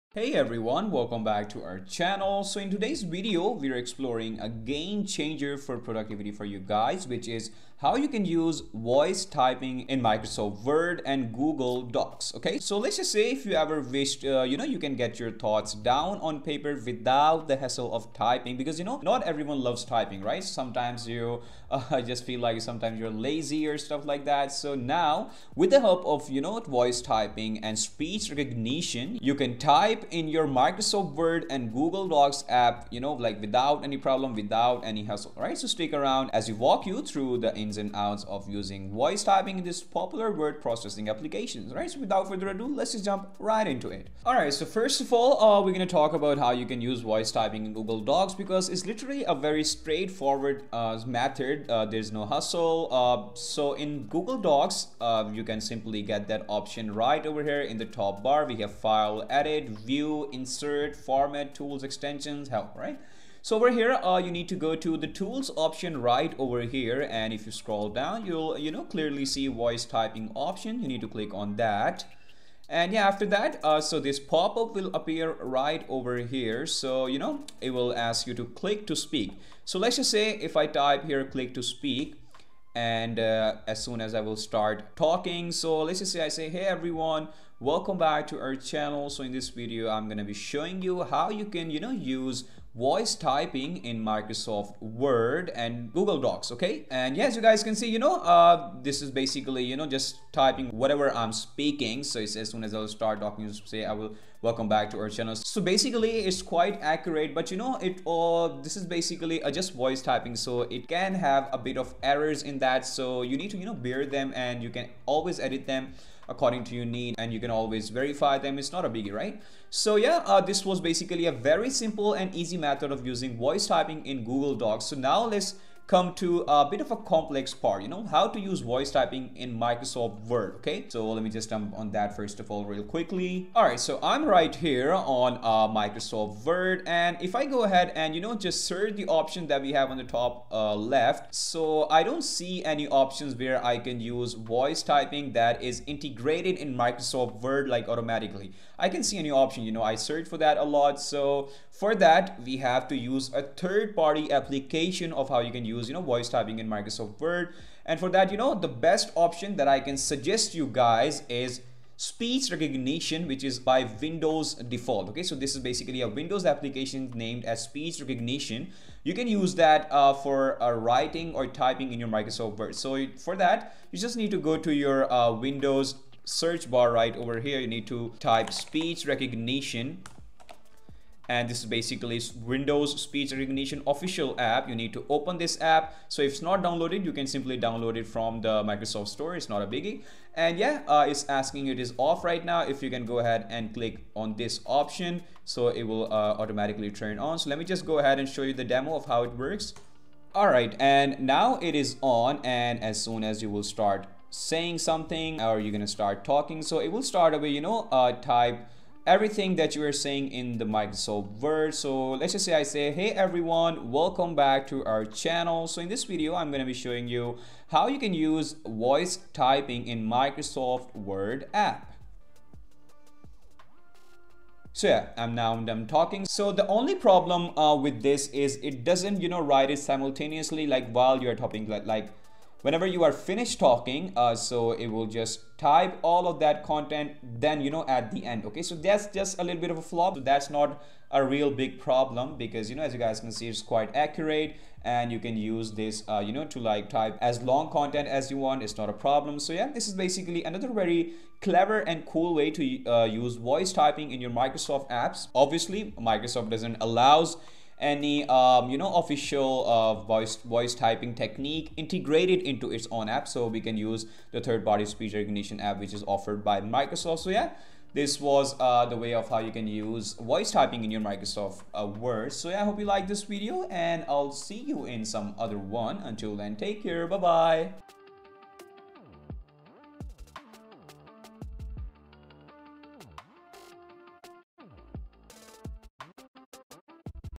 How To Use Voice Typing in Microsoft Word and Google Docs
In this video we will show you how to use Voice Tying Feature in Microsoft Word and Google Docs. This is a very important feature to use when trying to increase your writing speed. Voice typing allows you to effortlessly dictate text, offering a time-saving alternative to traditional keyboard input and enhancing the natural flow of your writing. The tutorial covers enabling voice typing, employing voice commands, and mastering the art of editing and formatting your text using your voice.